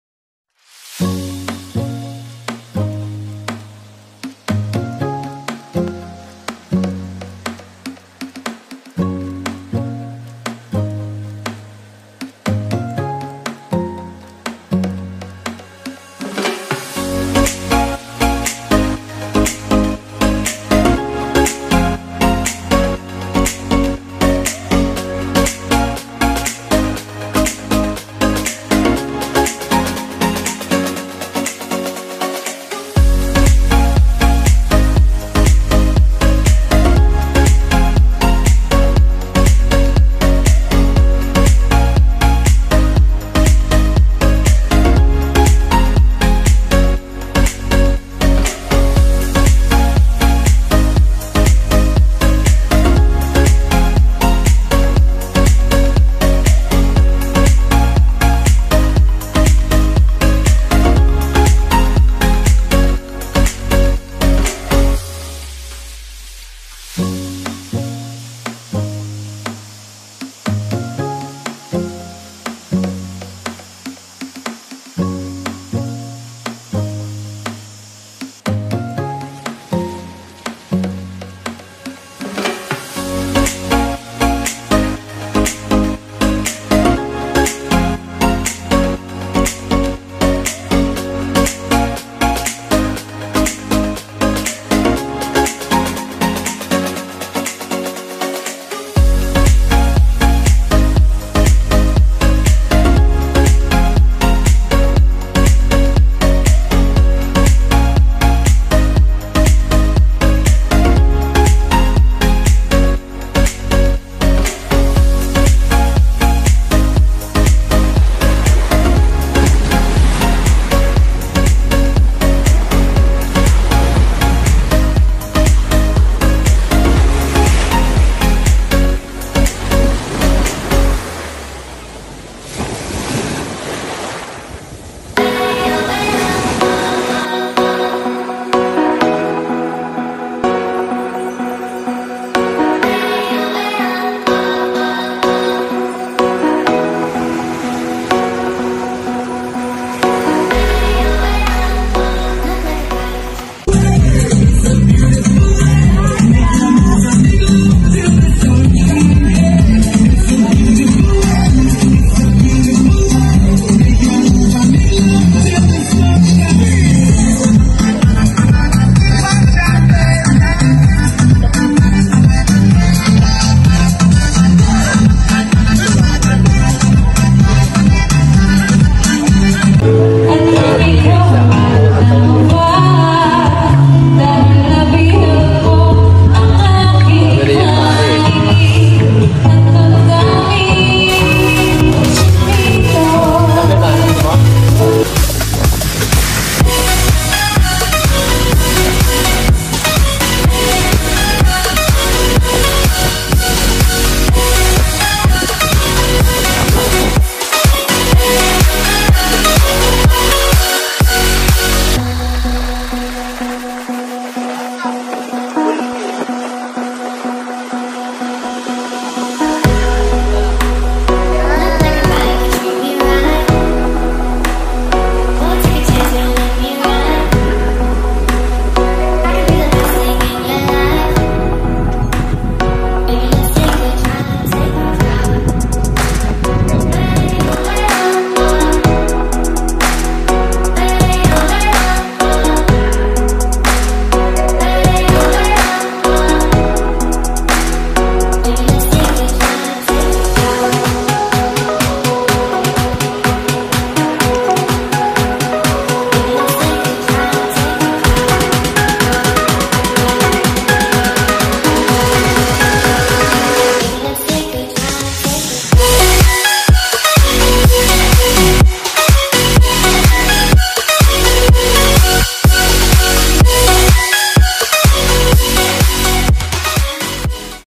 Boracay vacation with family 1st travel vlog
Hi guys this is my first vlog sorry for the quality of the video i dont have a camera for vlogging because this is my first time